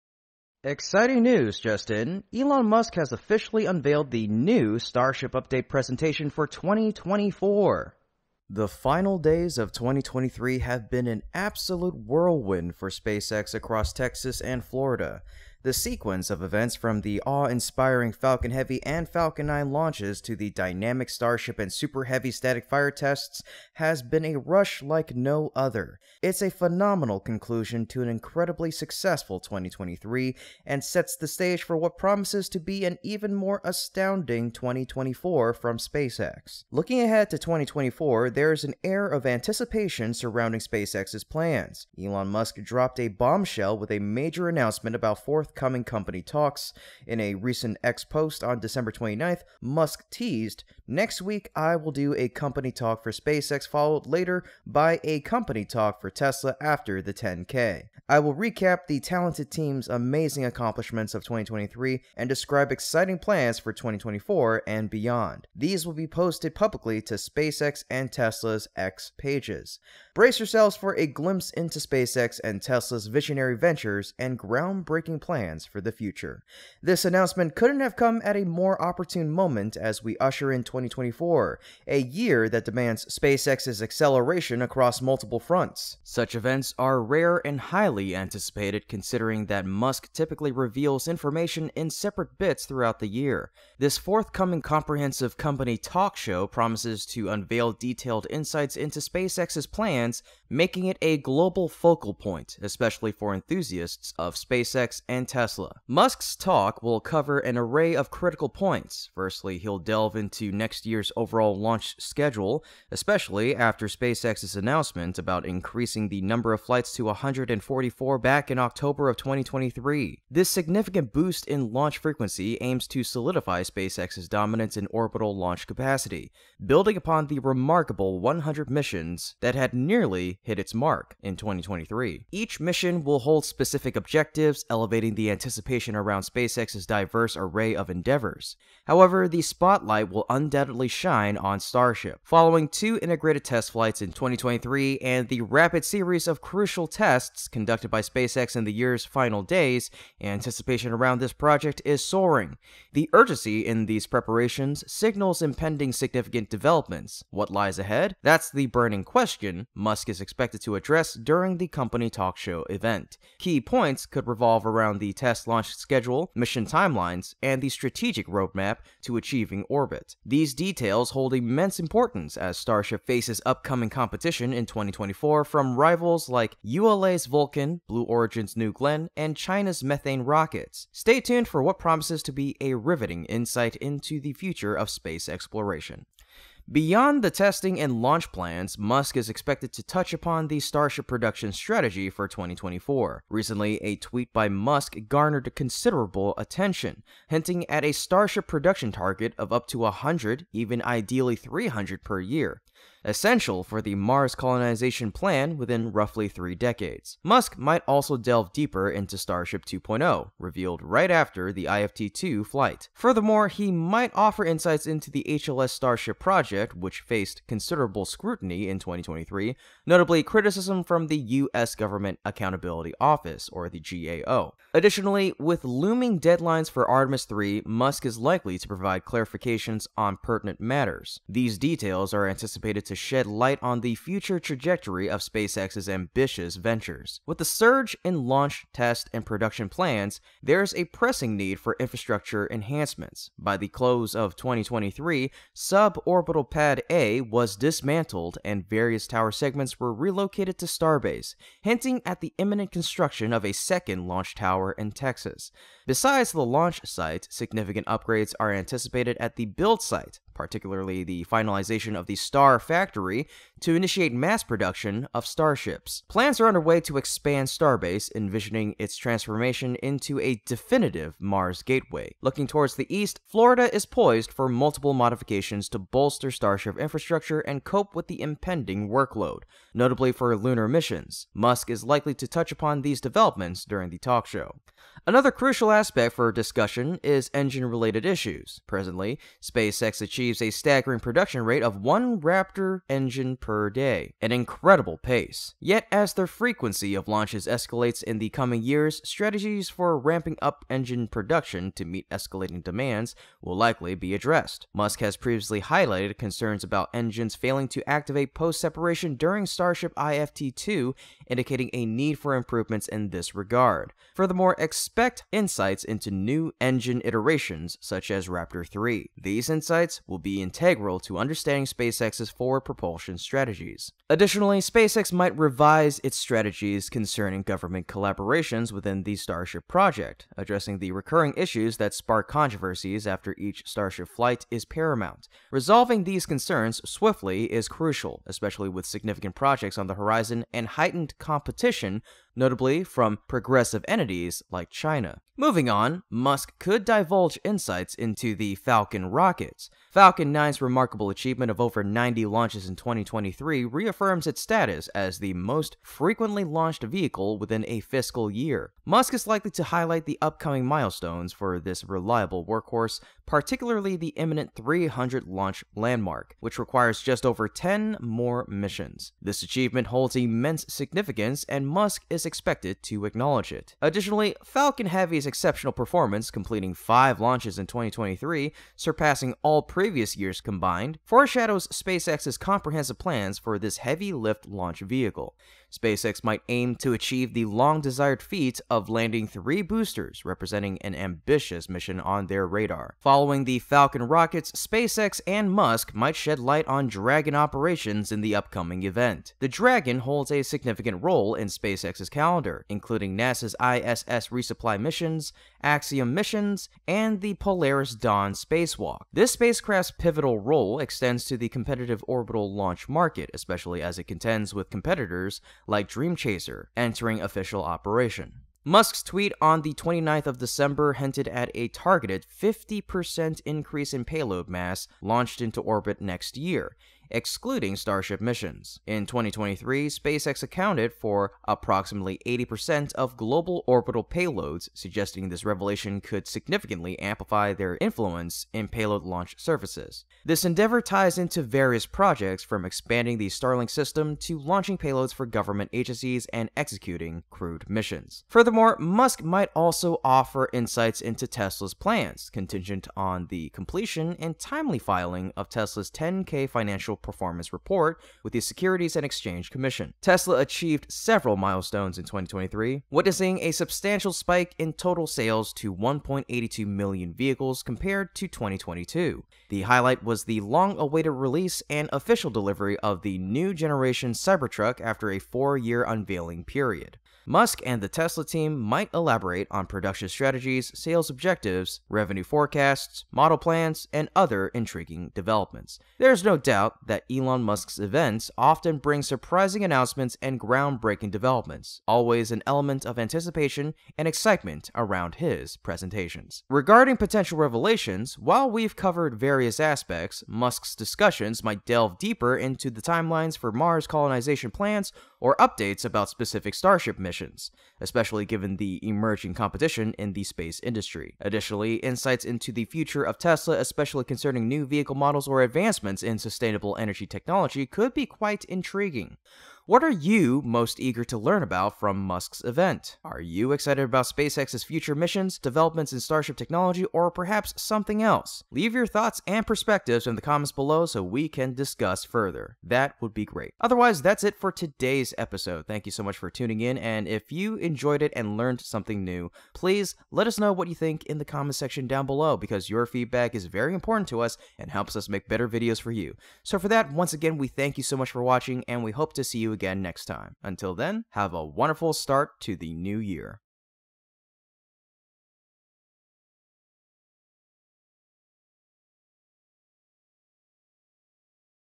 Elon Musk just officially revealed the NEW Starship Update presentation 2024...
Sources of image & video:
The final days of 2023 have been an absolute whirlwind for SpaceX across Texas and Florida. The sequence of events, from the awe-inspiring Falcon Heavy and Falcon 9 launches to the dynamic Starship and Super Heavy static fire tests, has been a rush like no other. It's a phenomenal conclusion to an incredibly successful 2023 and sets the stage for what promises to be an even more astounding 2024 from SpaceX.
Looking ahead to 2024, there's an air of anticipation surrounding SpaceX's plans. Elon Musk dropped a bombshell with a major announcement about forthcoming company talks. In a recent X post on December 29, Musk teased, "Next week, I will do a company talk for SpaceX, followed later by a company talk for Tesla after the 10K. I will recap the talented team’s amazing accomplishments of 2023 and describe exciting plans for 2024 and beyond! These will be posted publicly to @SpaceX and @Tesla". Brace yourselves for a glimpse into SpaceX and Tesla's visionary ventures and groundbreaking plans for the future!
This announcement couldn't have come at a more opportune moment as we usher in 2024—a year that demands SpaceX's acceleration across multiple fronts. Such events are rare and highly anticipated, considering that Elon Musk typically reveals information in separate bits throughout the year. This forthcoming comprehensive company talk show promises to unveil detailed insights into SpaceX's plans, making it a global focal point, especially for enthusiasts of SpaceX and Tesla.
Elon Musk's talk will cover an array of critical points. Firstly, he'll delve into next year's overall launch schedule, especially after SpaceX's announcement about increasing the number of flights to 144 by October 2023. This significant boost in launch frequency aims to solidify SpaceX's dominance in orbital launch capacity, building upon the remarkable 100 missions achieved in 2023. Each mission will hold specific objectives, elevating the anticipation around SpaceX's diverse array of endeavors.